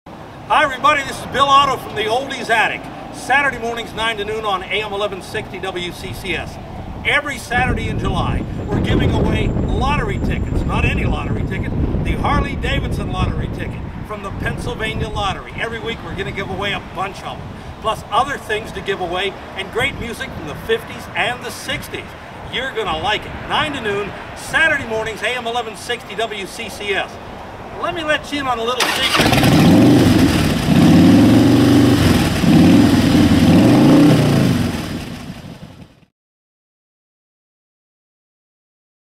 Bill and Harley
Bill Otto visited the Grapevine Restaurant in Indiana to talk about July 2012 on the Oldies Attic and the Harley Davidson PA Lottery instant ticket giveaway. The Oldies Attic airs each Saturday morning from 9 to noon on 1160 WCCS!